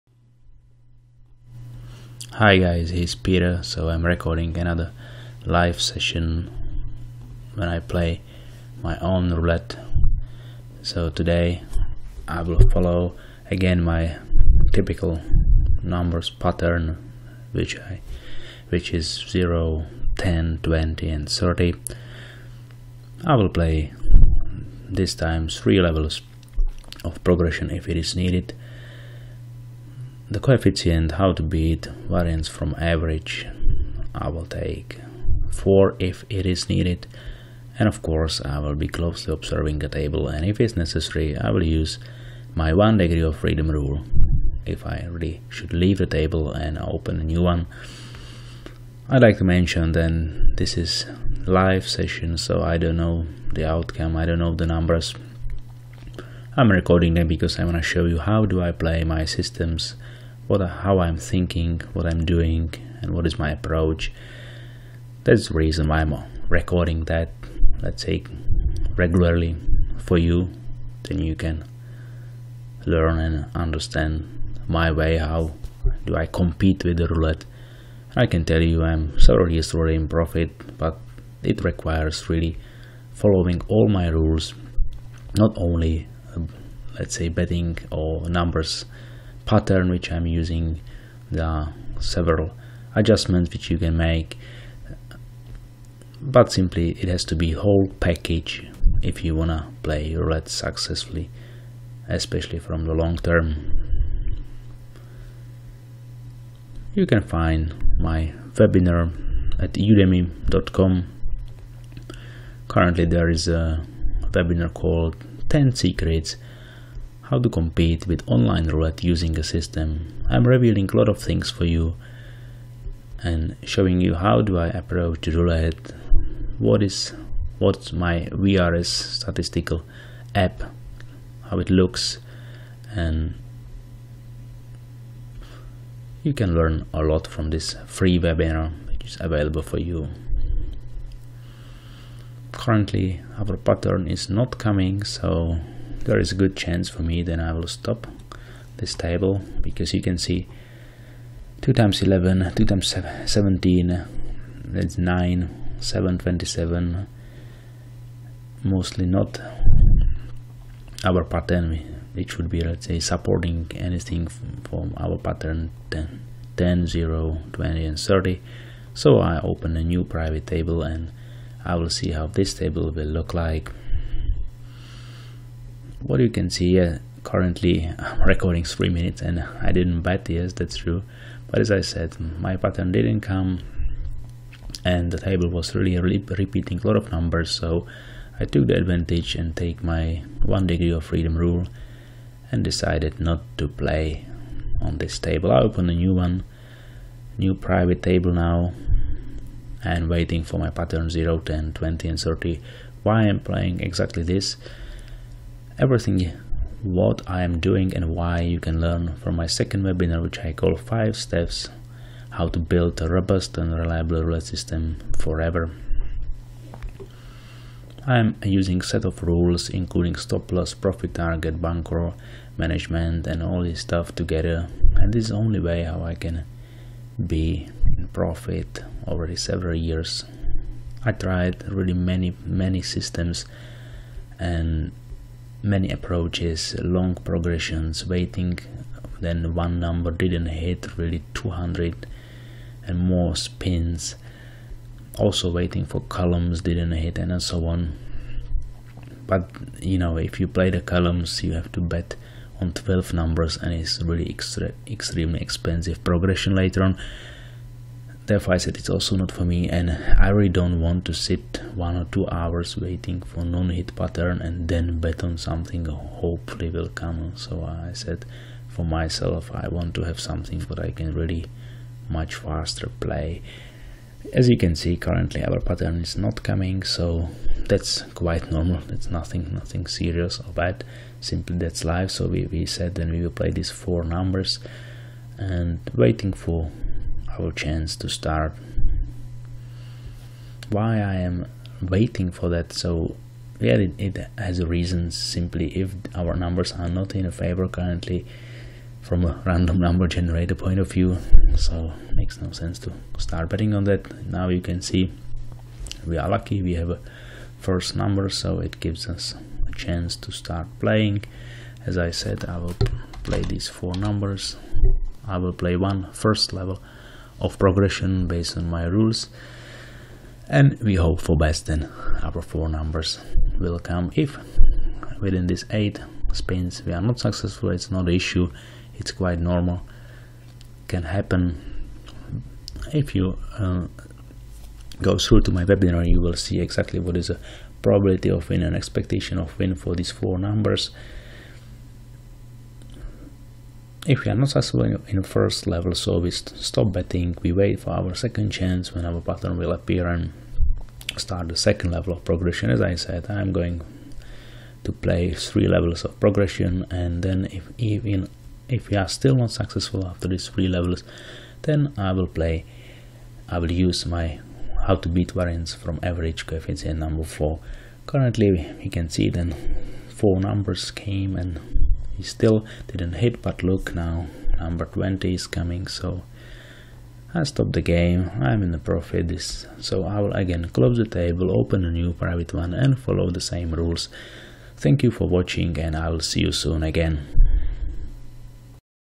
!LIVE! Online roulette session playing my own system (27-March-2017).
All roulette system rules I teach at my webinar "5 STEPS How to build robust and reliable roulette system, forever!". And I share with you all my knowledge and experience also in free webinar "10 Secrets How to compete with online roulette using a system"
My webpage , I am playing my own roulette system LIVE, again.
-Petr